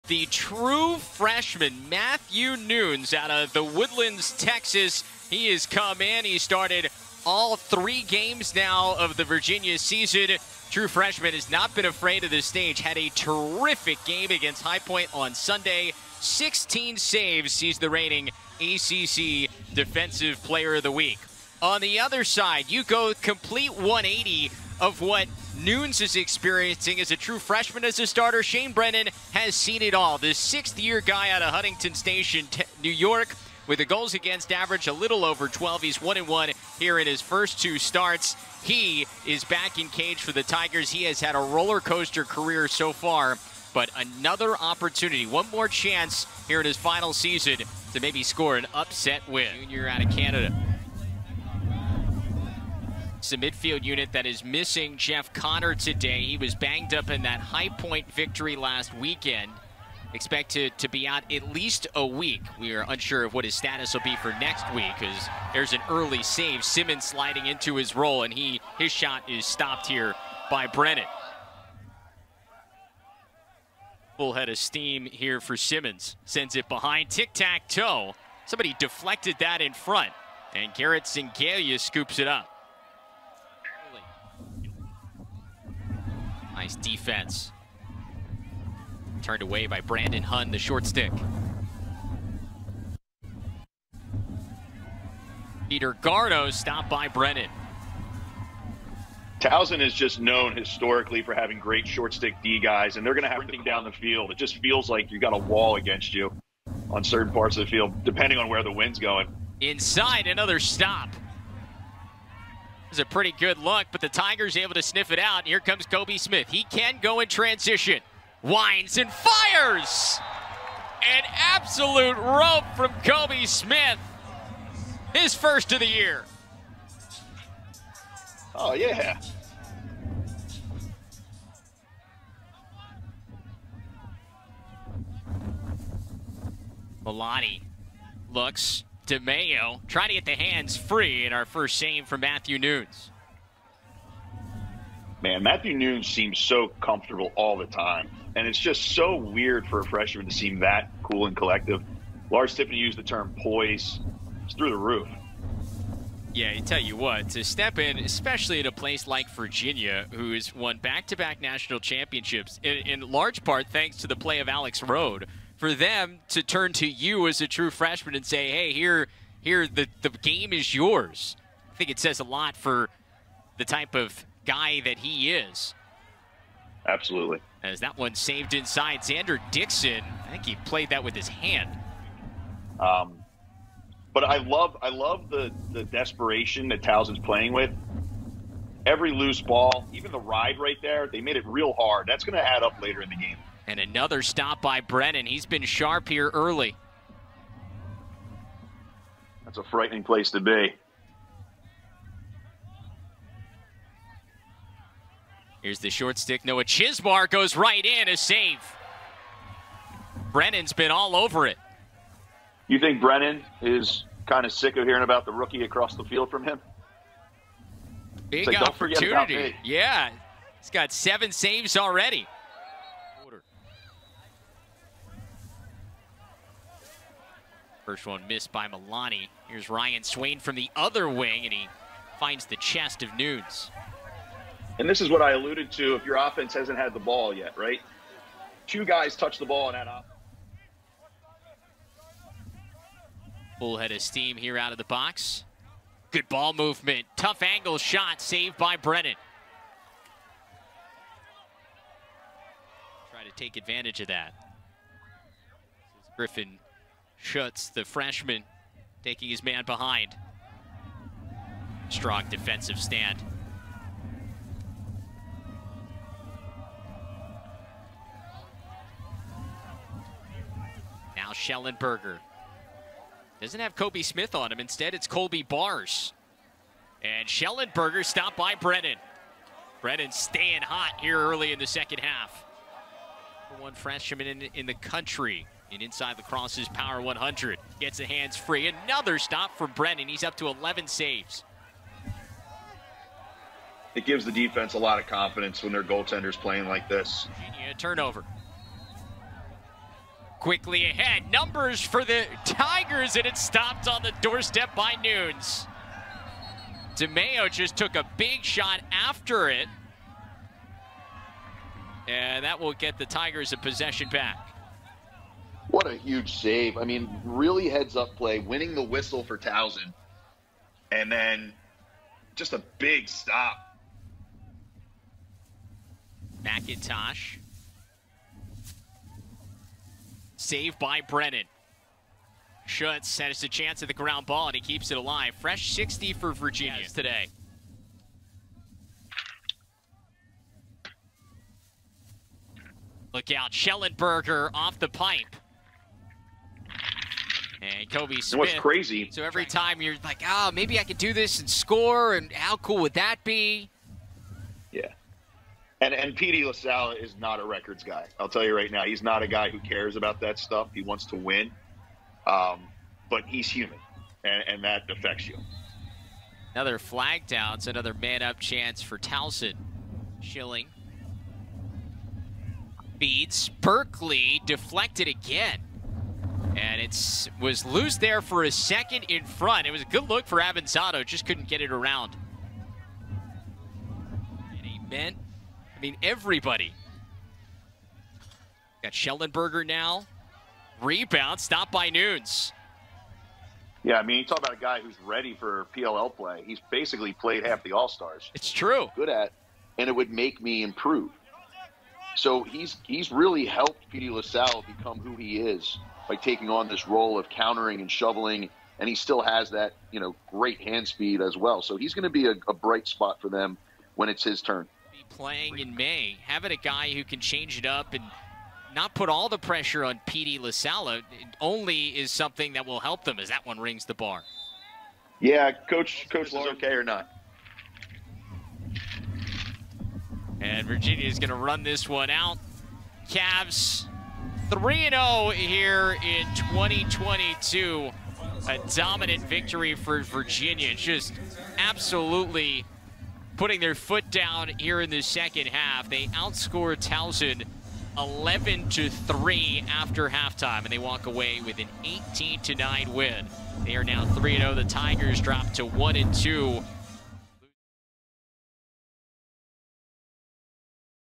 Matthew Nunes (Virginia) vs. Shane Brennan (Towson) - College Lacrosse Save Edit - 2/19/22
Highlights from the saves of the Virginia vs. Towson game.

Virginia goalie Matthew Nunes only needed to make 6 saves in the 18-9 Cavaliers win.

Towson goalie Shane Brennan played outstanding and had Towson looking like an upset was possible in the first half. Shane finished with 14 saves.

Looking for lacrosse goalie training? Checkout the 2-day virtual mini-camp I did with Tillman Johnson